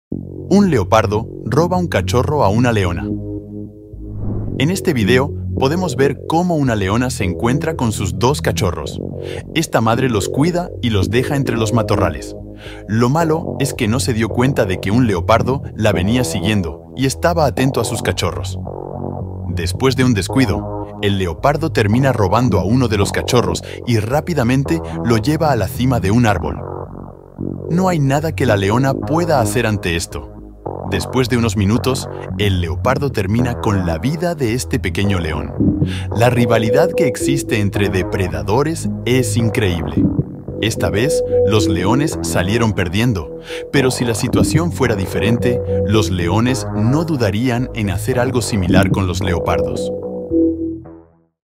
Leopardo Roba una Cría a una Leona
Rivalidad en la naturaleza: 🦁🐆 Una leona descuida a sus cachorros, y un astuto leopardo aprovecha la oportunidad para robar a uno de los pequeños felinos. A pesar del intento desesperado de la leona, el leopardo escapa y, trágicamente, pone fin a la vida del cachorro. 🌿🔥